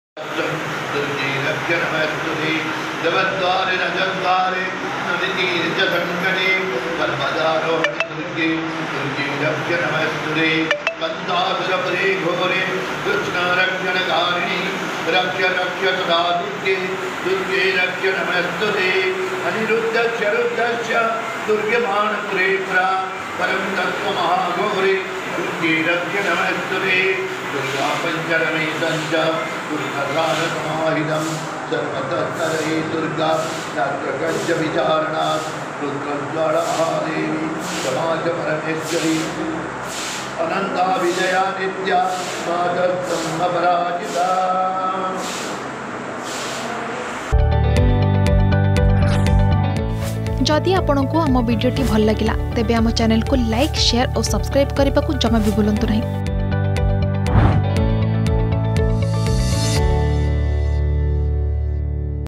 ବରିଷ୍ଠ ପୋଲିସ ଅଧିକାରୀଙ୍କ ମୁହଁରୁ ବାହାରୁଛି ଅନର୍ଗଳ ମନ୍ତ୍ର | Mantra is coming out senior police officer
ଜଗତସିଂହପୁରର ନୂଆ ଏସପି ଭାବେ କାର୍ଯ୍ୟଭାର ଗ୍ରହଣ କରିଛନ୍ତି ଭବାନୀ ଶଙ୍କର ଉଦ୍ ଗାତା । ଶାରଳଙ୍କ ପୀଠରେ ପୂଜାର୍ଚ୍ଚନା କରିବା ସହ ଉଚ୍ଚାରିଥିଲେ ଅନର୍ଗଳ ମନ୍ତ୍ର ଯାହାକୁ ଦେଖି ସେବାୟତ ଓ ପ୍ରଶାସନିକ ଅଧିକାରୀ ମାନେ ଆଶ୍ଚର୍ଯ୍ୟାନ୍ଵିତ ହୋଇଥିଲେ ।